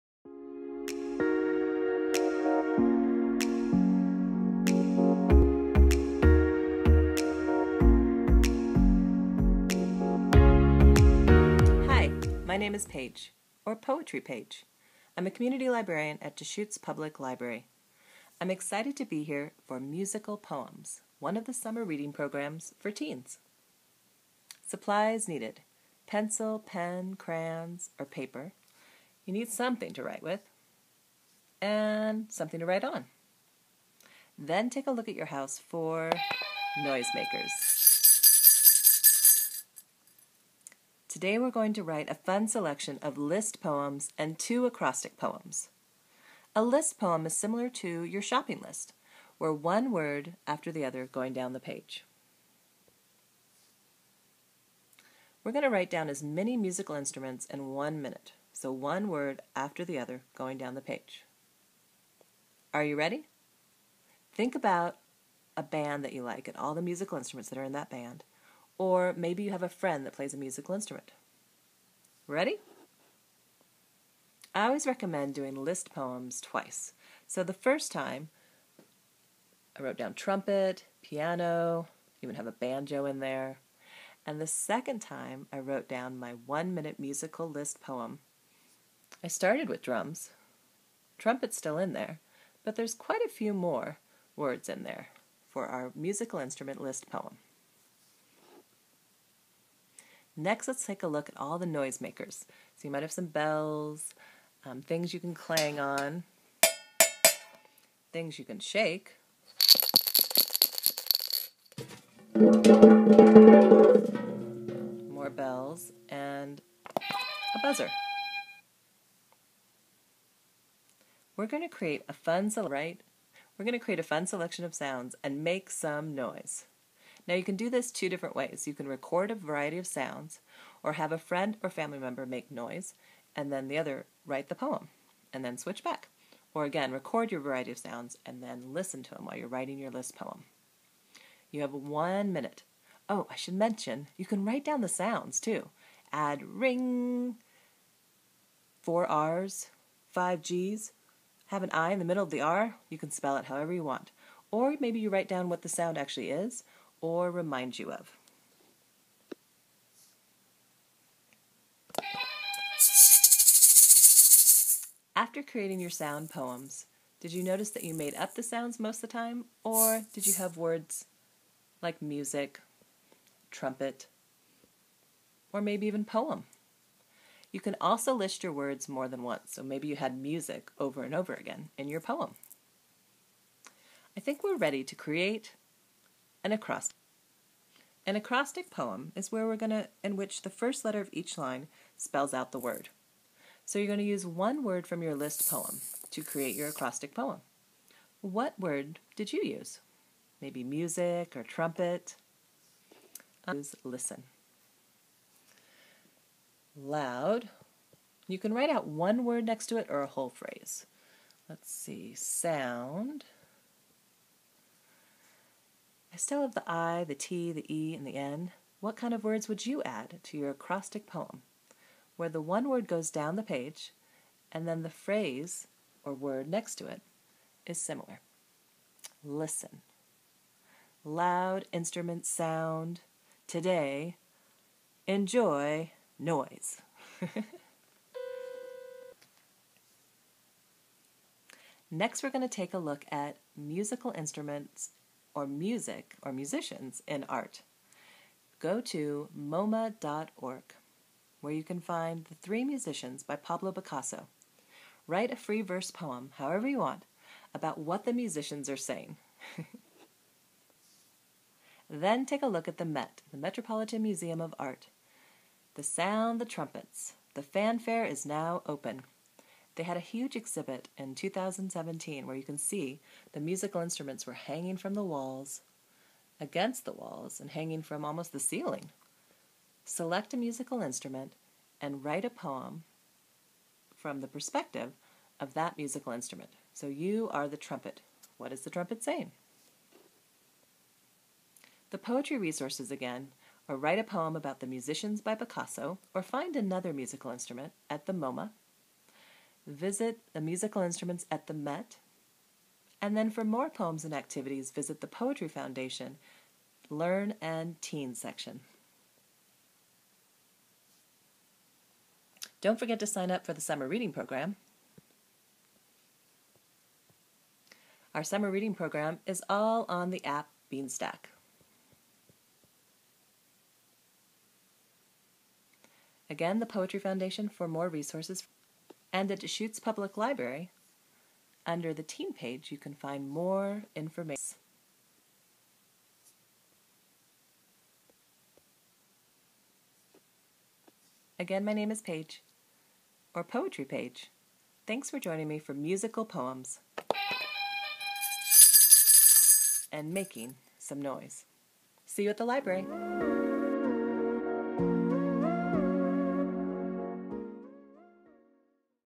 Musical Poems
Make some noise! Listen to a variety of sounds and music to create your own acrostic and list poems. To participate in this project you will need:

- Pencils or other coloring supplies of your choice
- Paper
- One object that makes noise (for example: a whistle, a box of cereal you can shake, etc.)